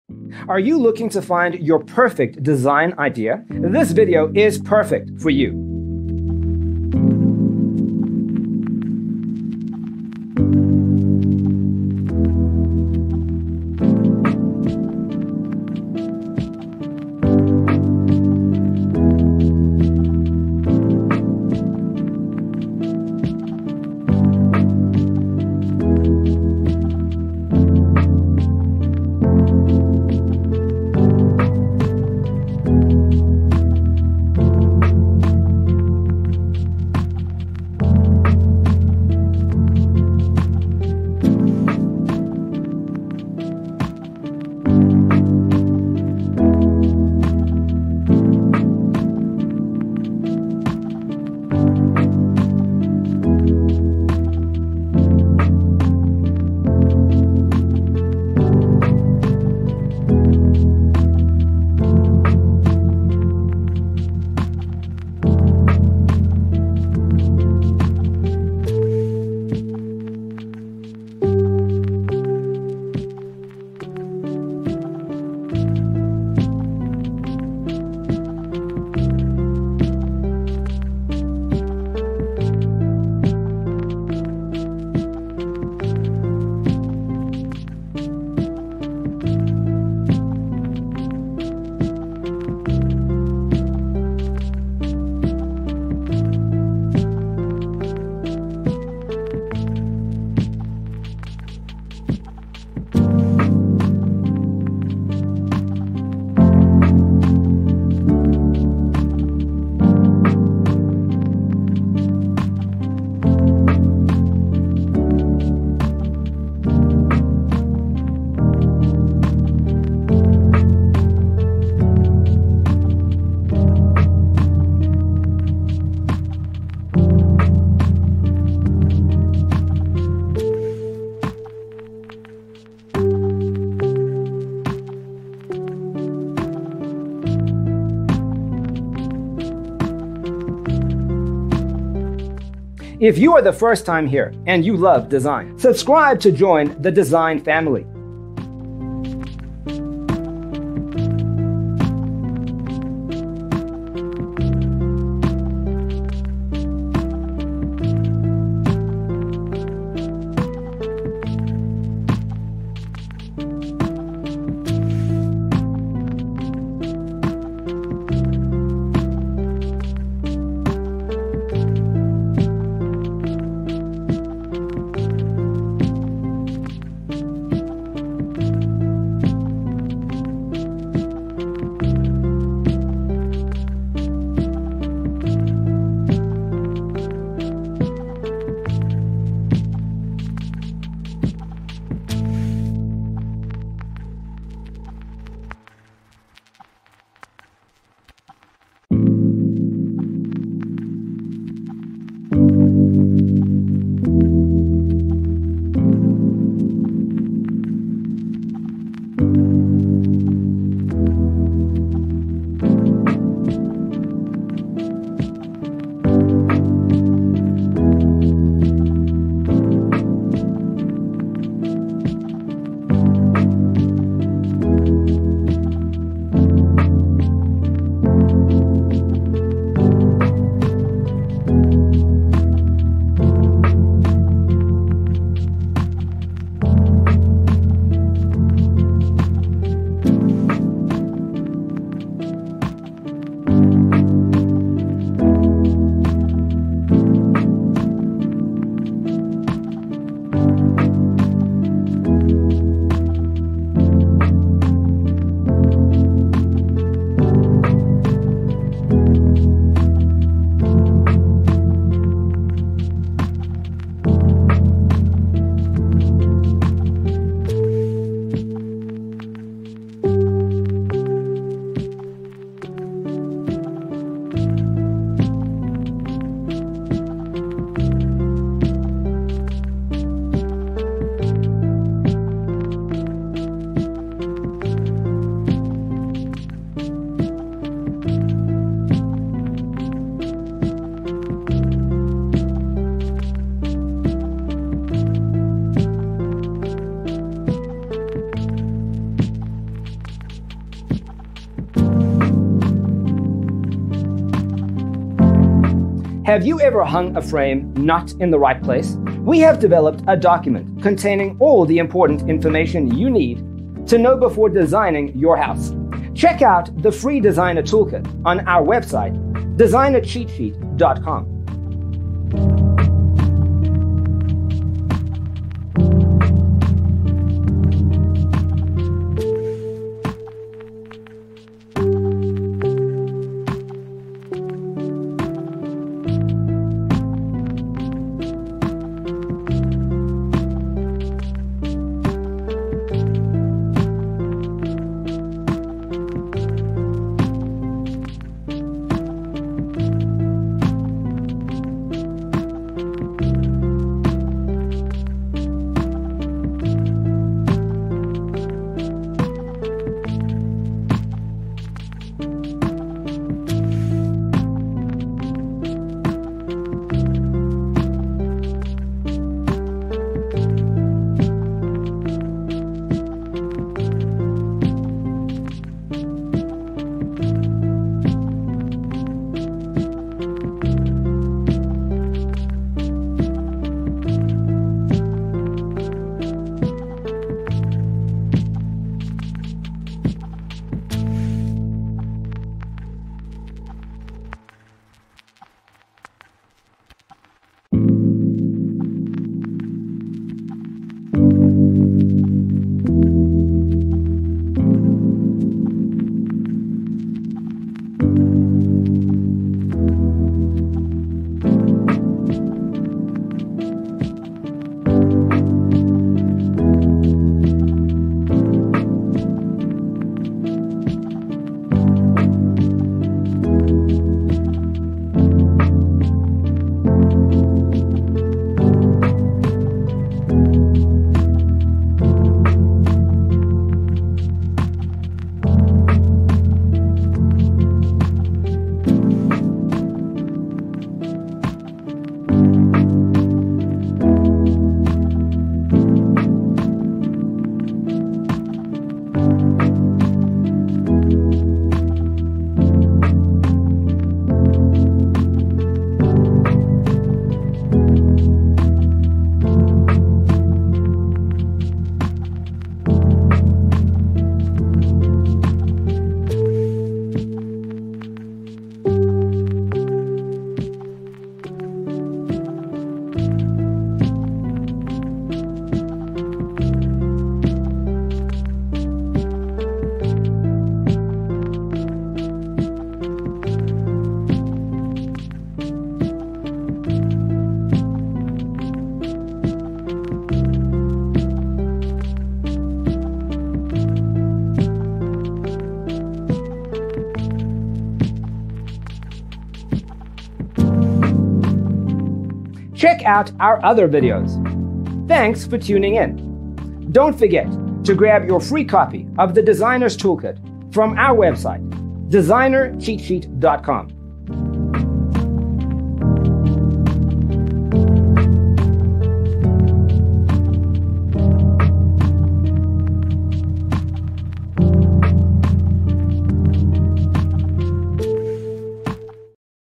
75 Painted Wood Floor And Cork Floor Home Gym Design Ideas You'll Love 🔴
0:00:00 - Intro
0:00:12 - Painted Wood Floor And Cork Floor Home Gym Design Ideas You'll Love: Inspirations 1 to 25
0:03:03 - Painted Wood Floor And Cork Floor Home Gym Design Ideas You'll Love: Inspirations 26 to 50
0:06:07 - Painted Wood Floor And Cork Floor Home Gym Design Ideas You'll Love: Inspirations 51 to 75